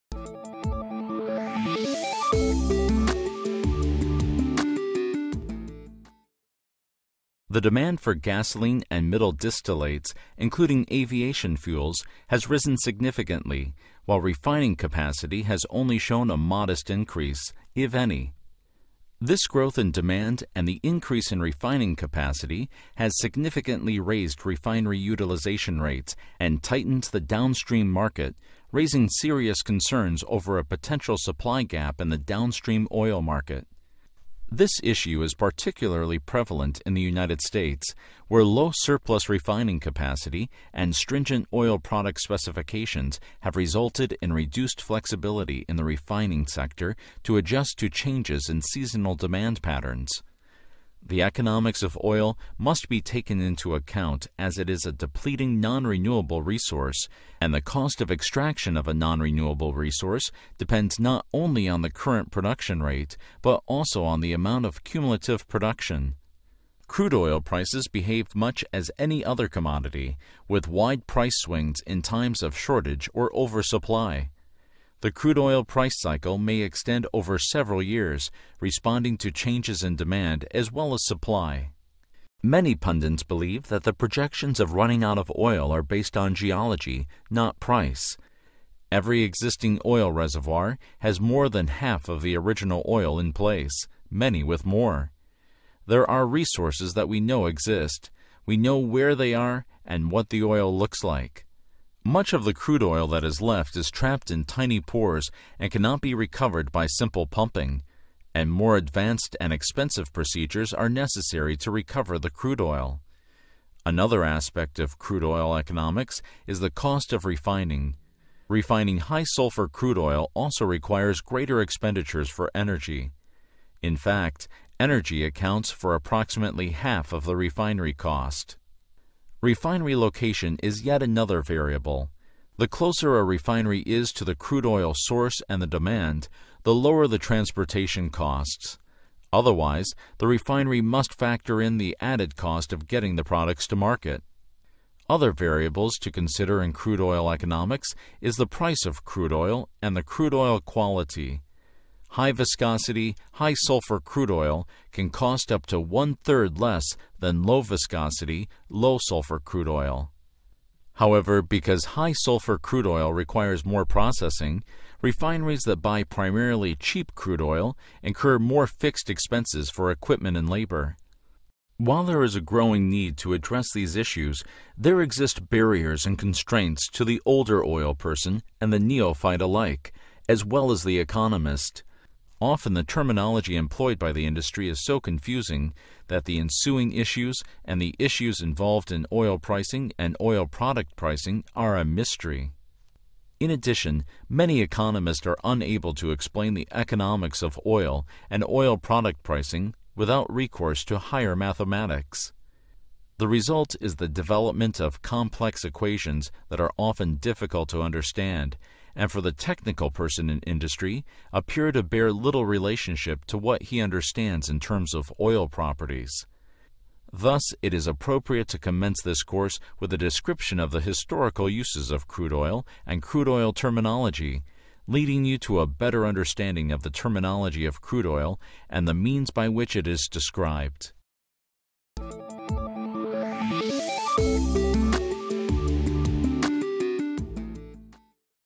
The Economics and Politics of Petroleum
The economics of oil must be taken into account, as it is a depleting and non-renewable resource and the cost of extraction of a non-renewable resource depends on production rate and amount of cumulative production.

This RedVector course,The Petroleum Industry - History, Terminology, and Culture will take a look into petroleum's verbal roots, it's initial uses and it's role in society today.